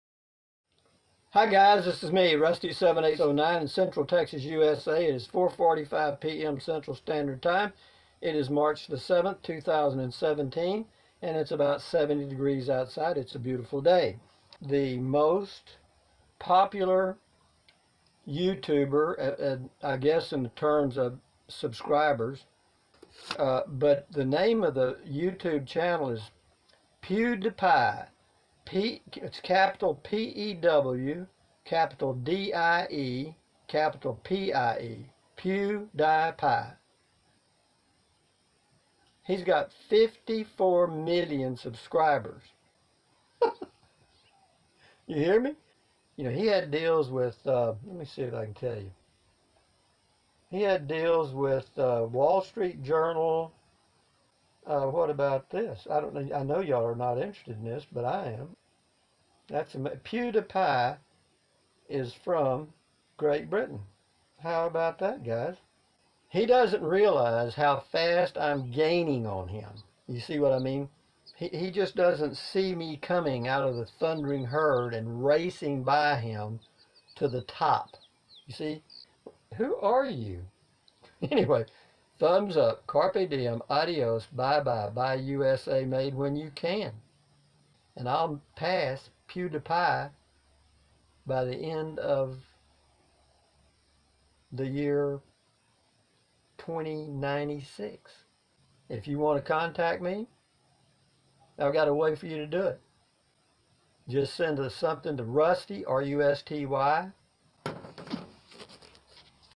Proof PewDiePie Was Working With The WSJ
this is definitive proof that the WSJ scandal was an inside job by bush and trump, just go to 0:46, someone tell philly d now